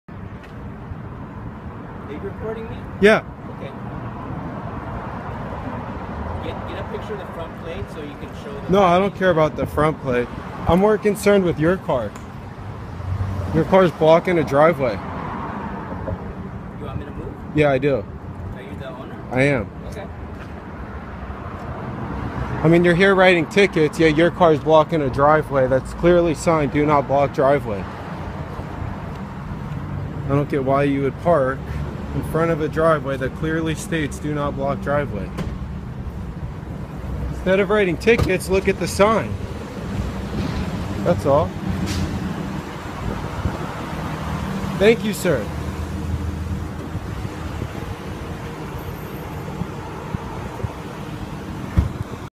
Parking enforcement tickets another car while blocking someones private driveway..
Meter maid was illegally blocking someone's driveway while riding tickets to random cars before I asked him to move his car. I wish those who wrote the tickets and enforced the laws and rules, also followed them.

Support the mrcheckpoint movement with as little as $1 a month and get real-time DUI Checkpoint alerts..